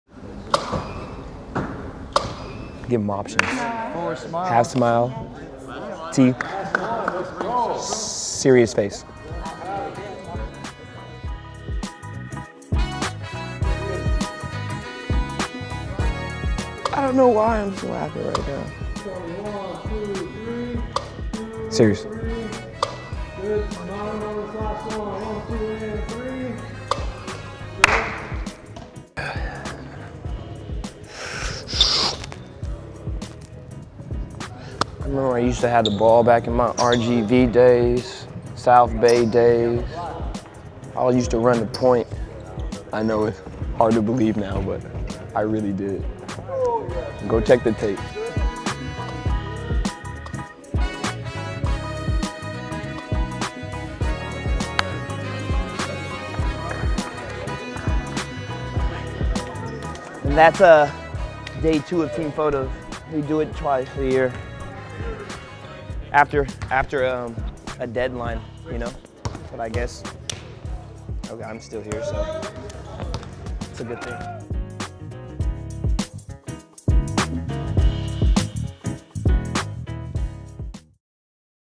Gary Payton II Mic'd Up at Golden State Warriors Picture Day
Gary Payton II mic'd up at Warriors team photo day.

Need we say more? 🎙️